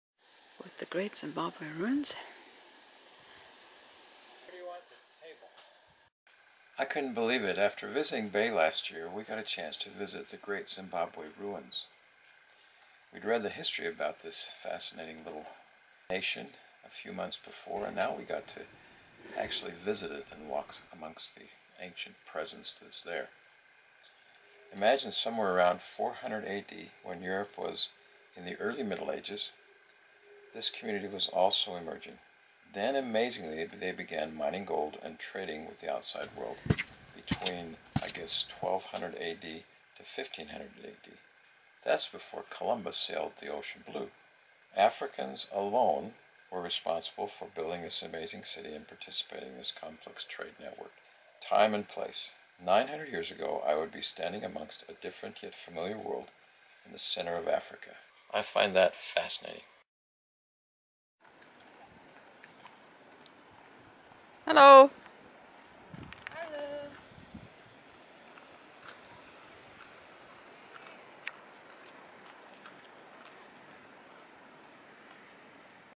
1200ad Surprise - African Nation Great Zimbabwe Ruins
These ancient native Africans were mining and trading Gold with the outside world 300 years before Columbus sailed.
Join us.. Travel the African bush in our old Land Rover, see amazing wild animals and birds, meet dedicated conservationists and experience a few crazy adventures along the way.

Filmed with a Sony Cybershot